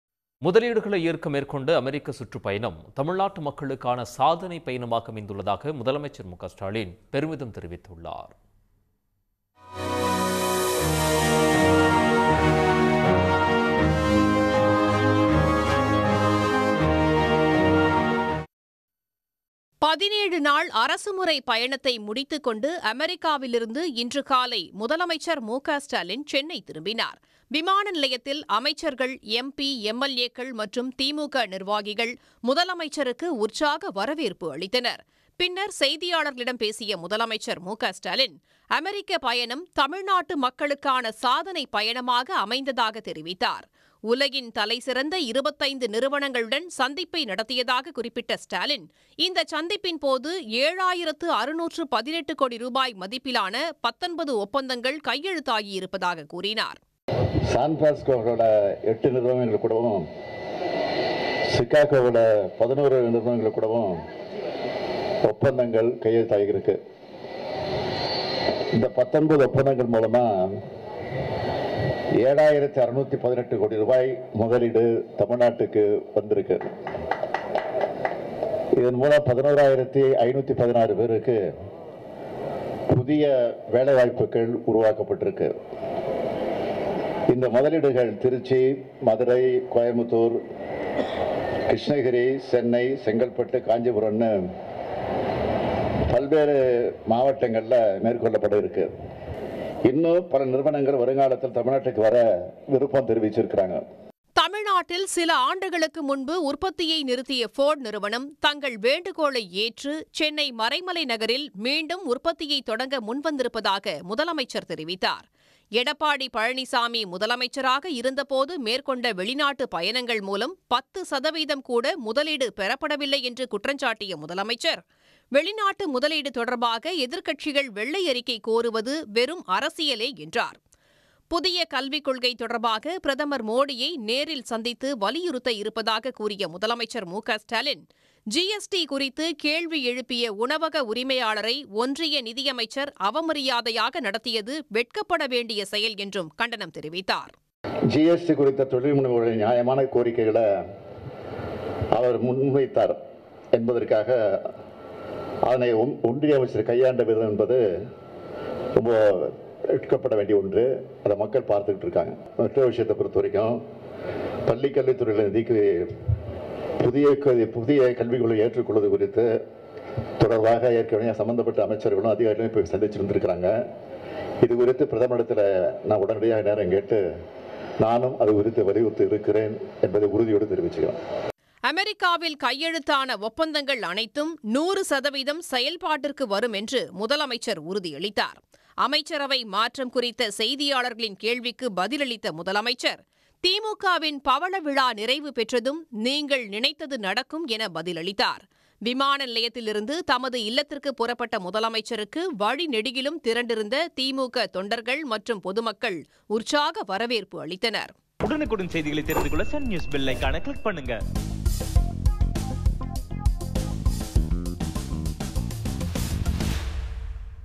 CM Stalin Says 19 MoUs Signed For Investment Of Rs 7,618 Cr During His US Visit | Sun News
அமெரிக்காவில் 7,618 கோடிக்கு 19 ஒப்பந்தங்கள்.. 11,500 பேருக்கு வேலைவாய்ப்பு.. 'இது ஒரு சாதனை பயணம்' - முதலமைச்சர் பேச்சு!